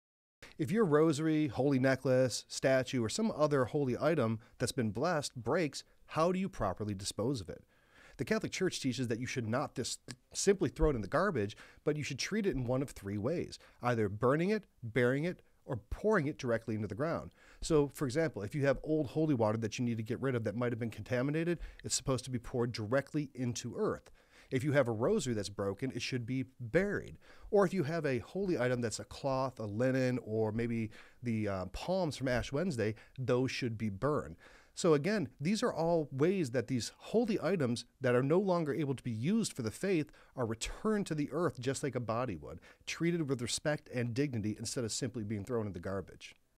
DON'T Throw Away Your Broken Rosary or Crucifix!!!
What should you do with a broken rosary or Holy item? Don't throw them away and do this instead.

"Sacred objects, which are designated for divine worship by dedication or blessing, are to be treated reverently and are not to be employed for profane or inappropriate use even if they are owned by private persons.” – Code of Canon Law 1171

In showing proper reverence, Catholic tradition simply states blessed items that are broken or damaged should be burned or buried. After an item has been burned, its ashes should also be buried. If the item does not disintegrate into the Earth, such as plastic, it is acceptable to break apart the item then discard it appropriately. In the case of votive candles, if it is safe to do so they should be burned completely.

The tradition of returning blessed items to the Earth stems from the idea that an object sanctified in God’s name should be returned to God, much like the way a person is buried and returned to the earth.

While the tradition of returning objects to the earth stems back thousands of years, in 1874 the Sacred Congregation for the Rites and the Holy Office issued formal determinations on what proper disposal methods are for blessed items. Any linen, vestment, or altar cloth should be burned and the ashes buried. Excess or contaminated Holy Water should be poured directly into the ground. Palms should be burned, and the ashes used for Ash Wednesday with the excess buried. Similarly, a rosary or statue would be buried.